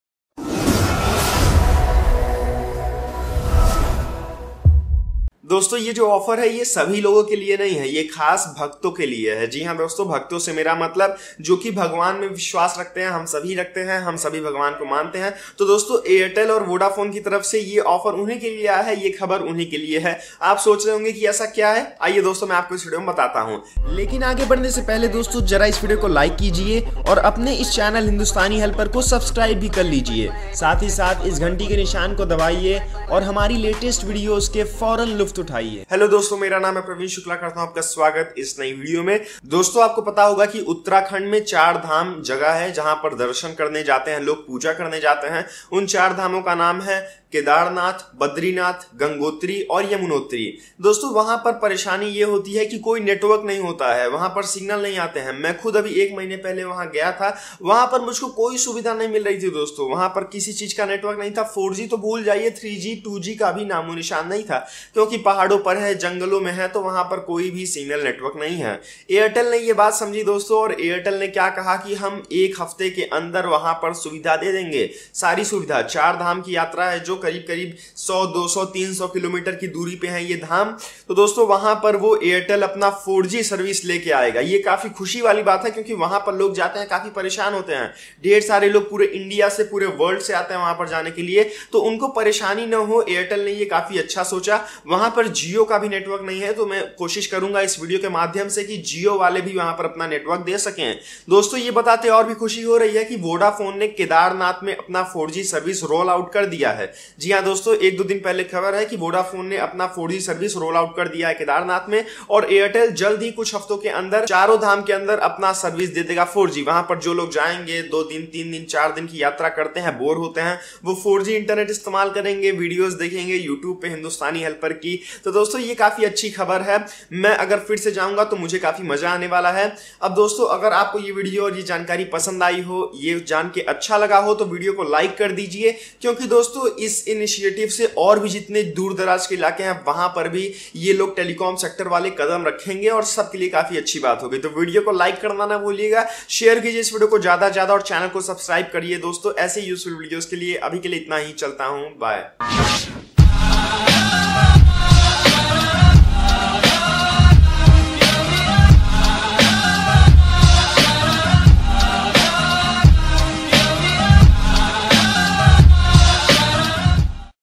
मानो चाहे न मानो - ऐसे ही है ये खबर - AIRTEL & VODAFONE NEW VENTURES
Leading telecom operator, Bharti Airtel stated that it would ensure seamless connectivity and convenience for its customers visiting the Char Dham locations – Kedarnath, Badrinath, Gangotri and Yamunotri over the next few weeks. Airtel also reaffirmed that it’s the only private telecom operator to provide 4G/3G/2G connectivity at all the locations, enabling its customers to stay in touch all the time. The telco said it recently restored network connectivity at Kedarnath as part of the restoration efforts following the devastating floods in June 2013. It also added that the network teams have overcome rough terrain and harsh weather to ensure network connectivity at all the four locations and has also beefed up its network across Uttarakhand.
Vodafone has finally rolled out its 4G services in Kedarnath, Uttarakhand allowing over half a million pilgrims visiting the holy shrine each year experience the high-speed data services.

Vodafone has successfully installed the 4G site in the vicinity of the temple at the height of about 11,755 ft, enabling customers to experience seamless 4G in and around the temple premises. Vodafone is among the first private telecom service providers to launch 4G at Kedarnath. The launch has been timed with the annual Chardham Yatra which draws pilgrims across the country to the recently renovated shrine set in the picturesque landscape, in Rudraprayag district.